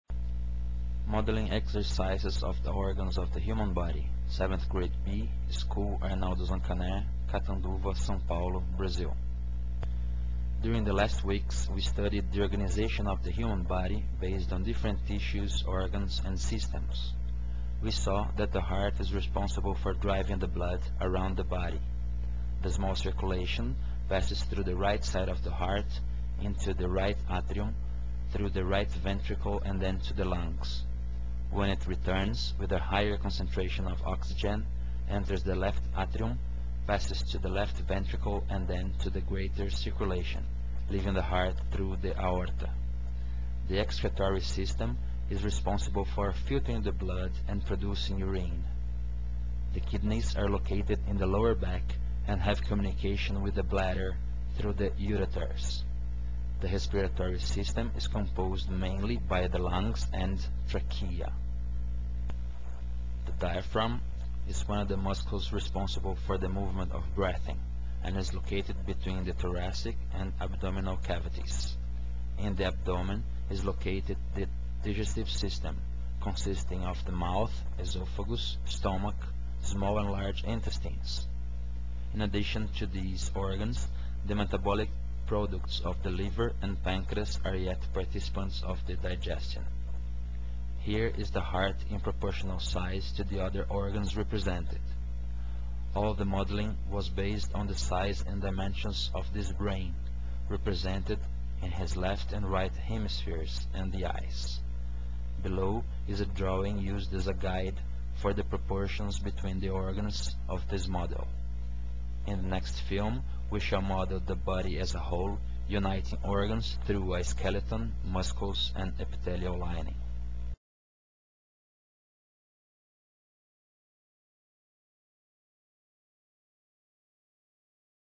Human Organs (in english)
Modeling exercise of the organs of human body
7th grade B - School Arnaldo Zancaner - Catanduva-SP - Brazil

During the last weeks, we studied the organization of the human body based on different tissues, organs and systems. We saw that the heart is responsible for driving the blood around the body. The small circulation passes through the right side of the heart, into the right atrium through the right ventricle and then to the lungs. When it returns, with a higher concentration of oxygen, enters the left atrium, passes to the left ventricle and then to the greater circulation, leaving the heart through the aorta.

The excretory system is responsible for filtering the blood and producing urine. The kidneys are located in the lower back and have communication with the bladder through the ureters. The respiratory system is composed mainly by the lungs and trachea.

The diaphragm is one of the muscles responsible for the movement of breathing and is located between the thoracic and abdominal cavities. In the abdomen is located the digestive system, consisting of the mouth, esophagus, stomach, small and large intestines. In addition to these organs, the metabolic products of the liver and pancreas are yet participants of the digestion. Here is the heart in proportional size to the other organs represented. All the modeling was based on the size and dimensions of this brain, represented in his left and right hemispheres and the eyes. Below is a drawing used as a guide for the proportions between the organs of this model.

In the next film, we shall model the body as a whole, uniting the organs through a skeleton, muscles and epithelial lining.